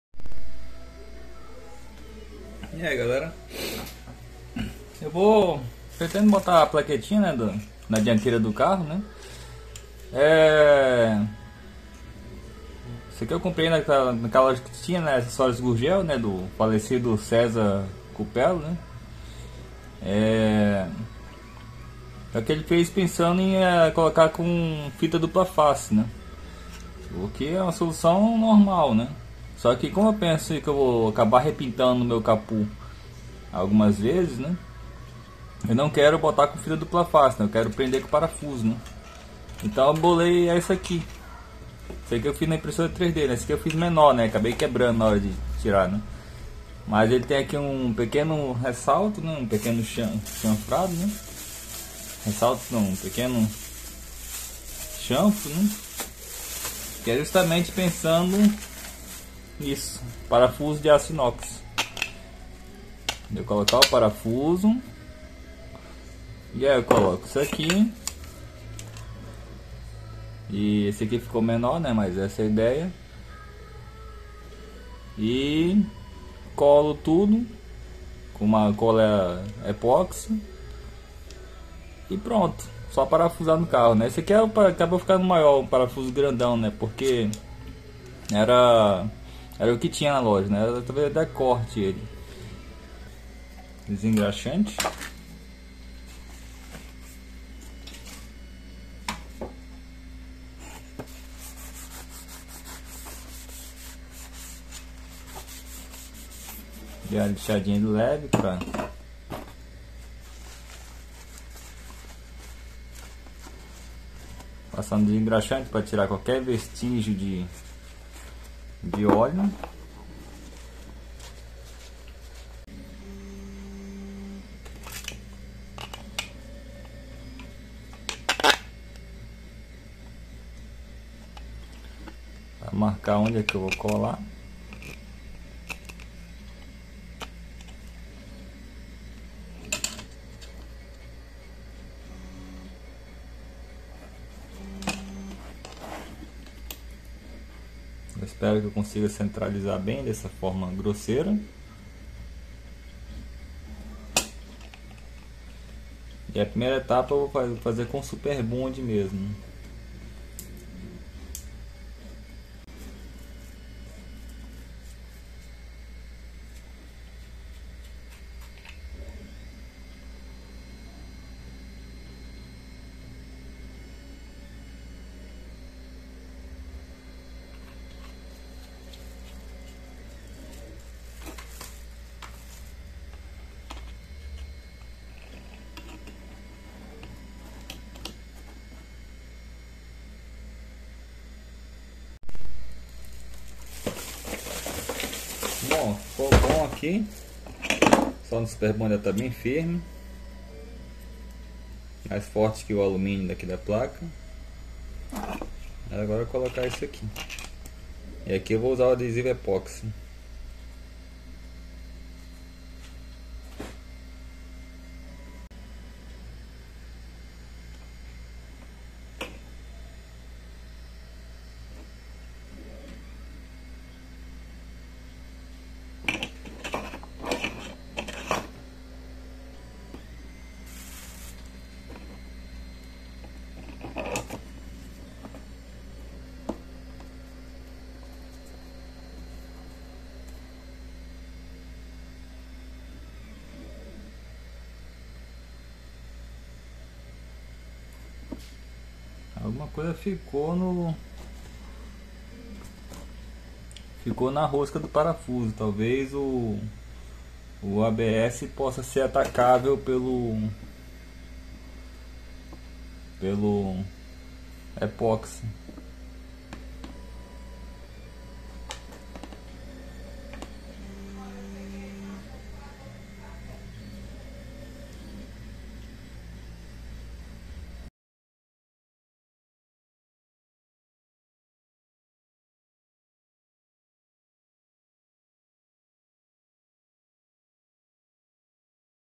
Colocando emblema Gurgel com parafusos
Acho que parafusos são uma solução melhor do que fita dupla face